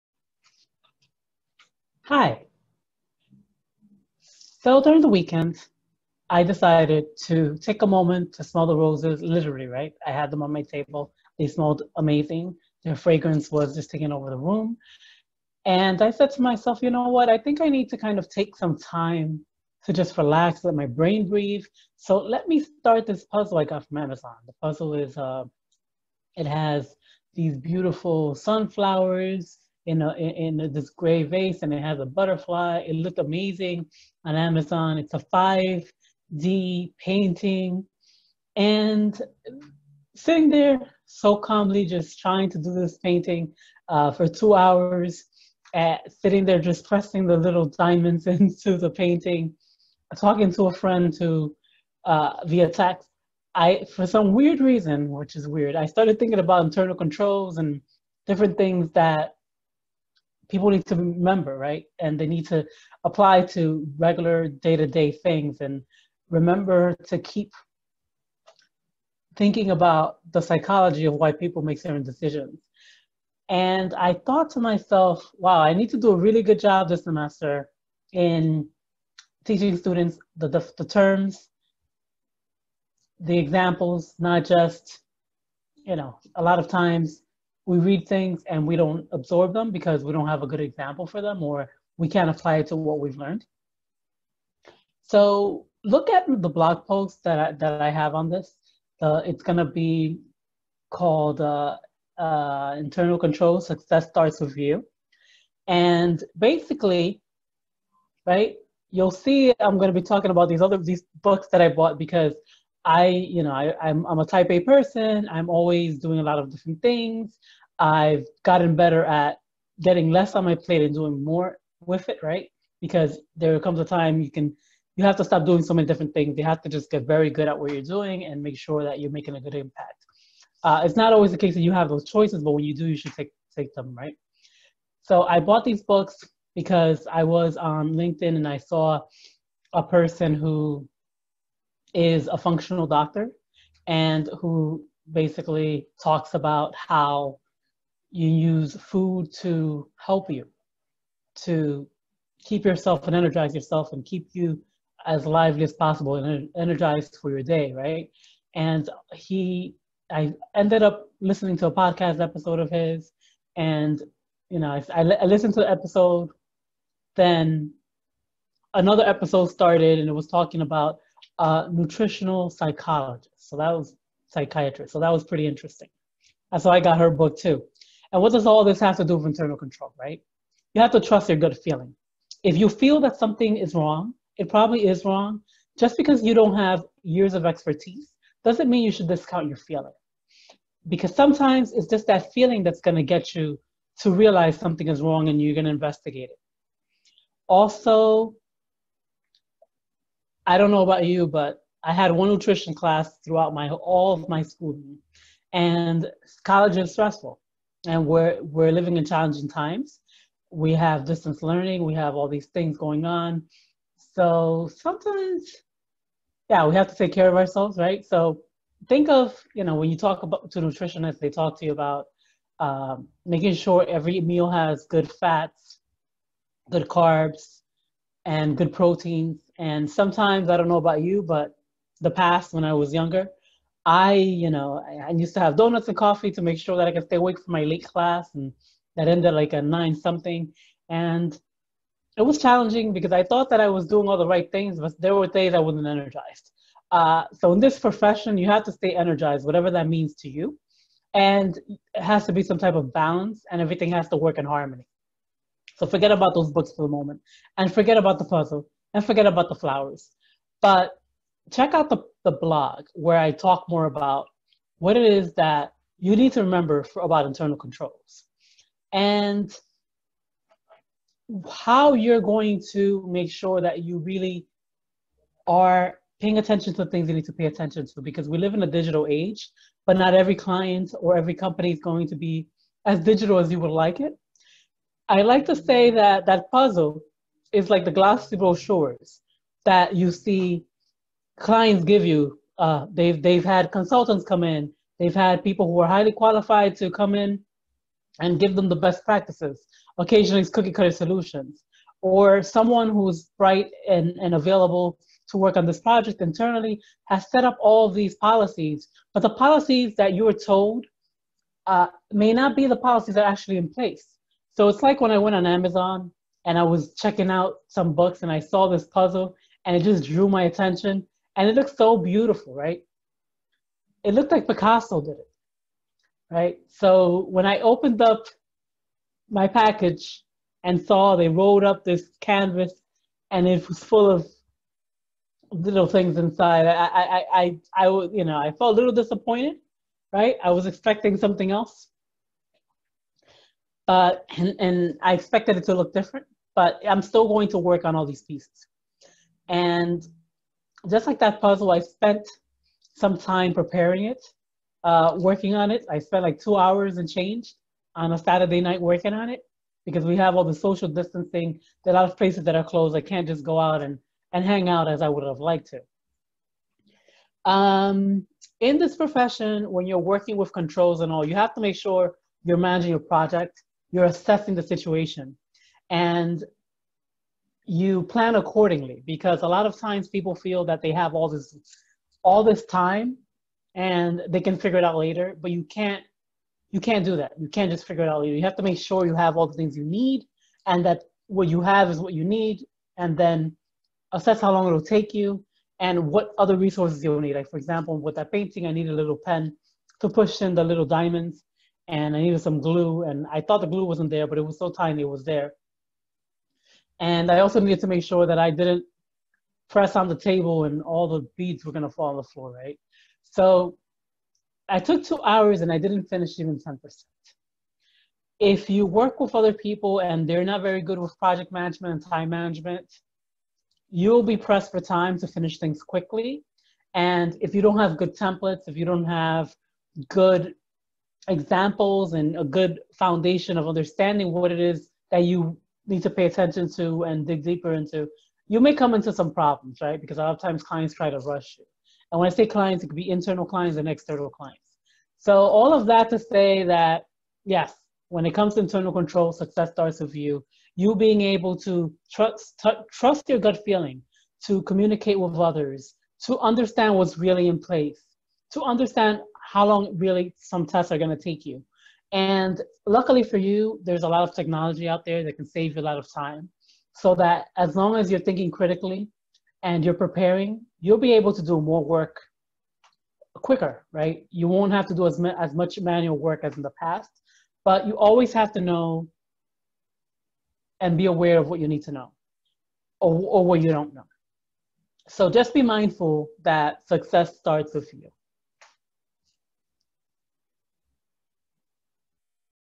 When it comes to internal controls, success starts with you
Trusting your gut feeling when you are starting out in the accounting profession